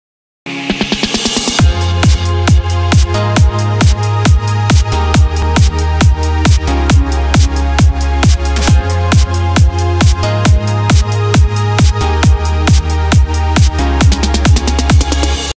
This is GlowZone360!
Featuring Glow-in-the-Dark Mini Golf, the latest technology in Laser Tag "ARENA X" (the first of its kind in Canada), interactive redemption arcades with FANTASTIC prizes and more!